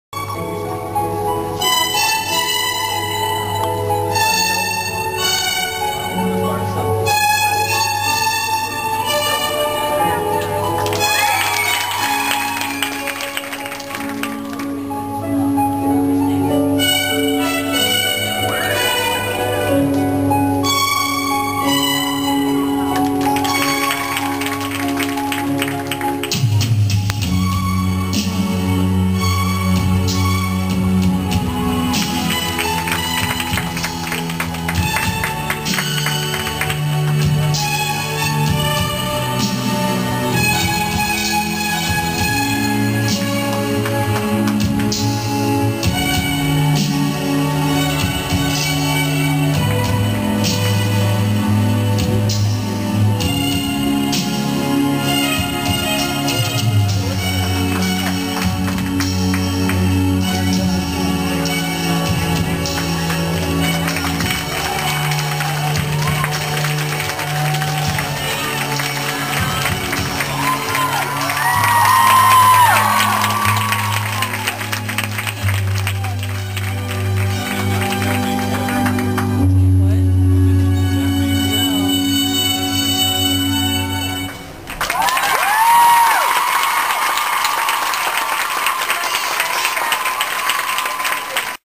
Galina Shirkina GALA 2006 LA Lights
Ukrainian Galina Shirkina performs her gala at LA Lights.